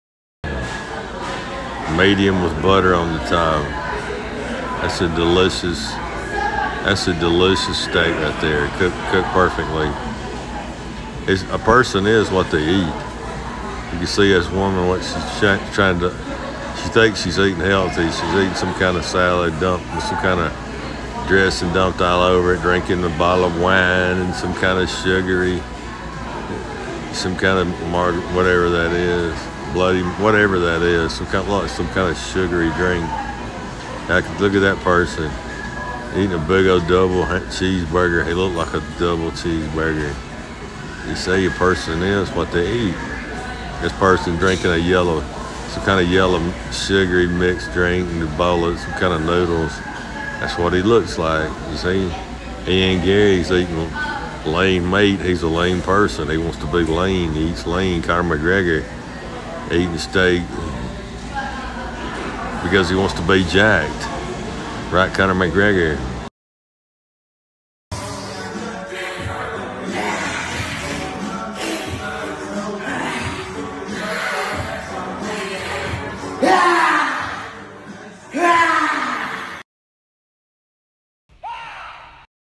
Conor McGregor Low Dose Peds Eats Steak To Get Jacked
Super Cash eats Large Ribeye medium with Real Butter , Double Broccoli with Real Butter , no potatoes , no bread , no sauces , 4-5 times per week to stay Jacked . High Fat , High Protein . Conor McGregor tries to act like a bad ass , but he’s simply a poser , an high level mixed martial artist must eat the right food , and avoid bad things to stay Jacked .Super Cash posts these videos about food ; because maybe a few Super Subscribers don’t know that eating is a part of getting Jacked .Numerous current studies show that dieters who follow high-protein low-carb strategies — even plans with higher fat intake — lose more fat and maintain or gain more muscle mass than dieters who rely on higher carb diets.